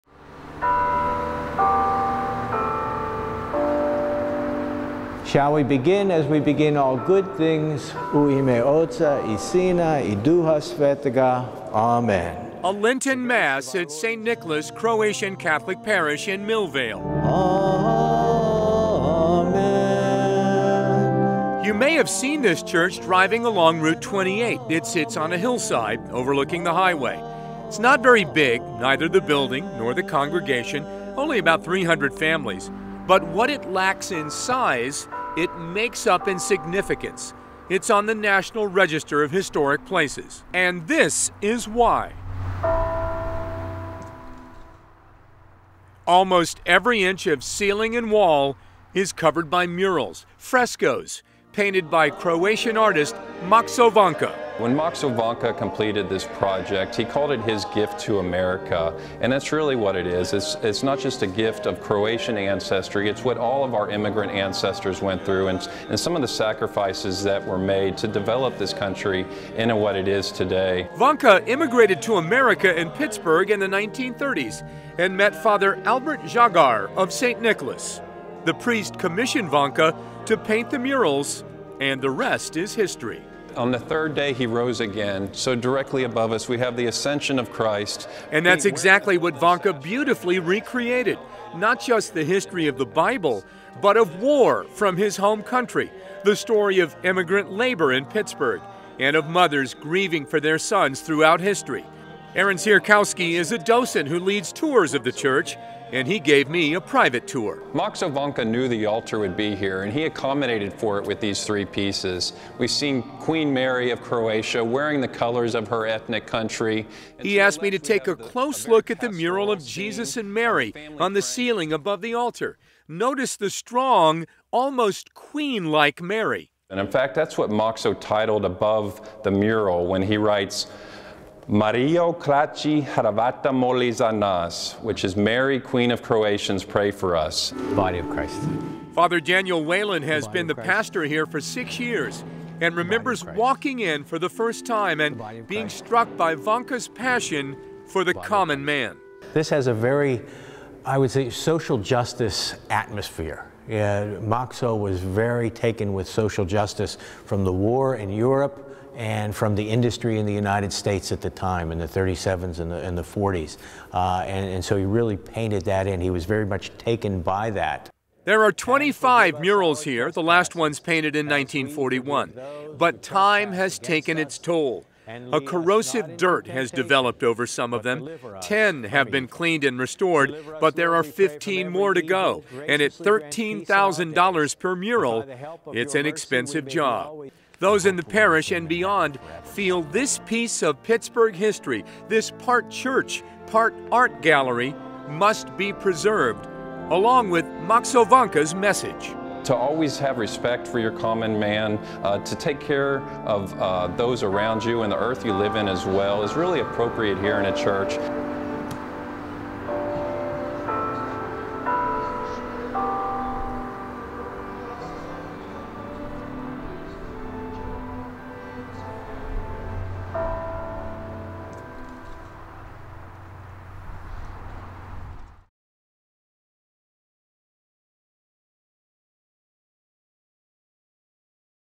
St. Nicholas Murals
St. Nicholas Croatian Catholic Church in Millvale, Pa. is on the national register of historic places.. WPXI TV Anchor David Johnson and Special Projects Photographer/Editor Tim Holoman take you on a journey of time and faith through the painted murals of Maxo Vanka.